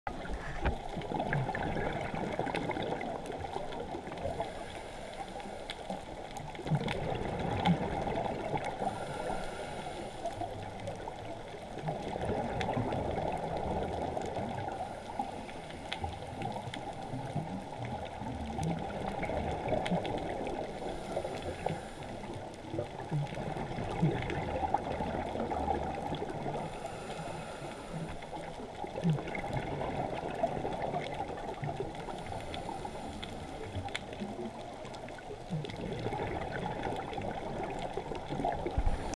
Playa Lagun: Porcupinefish hiding in barrel sponge
A little surprise when checking out a barrel sponge at Playa Lagun; Curaçao; 04/Aug/16

Filmed using GoPro Hero 3 with red filter on a stick.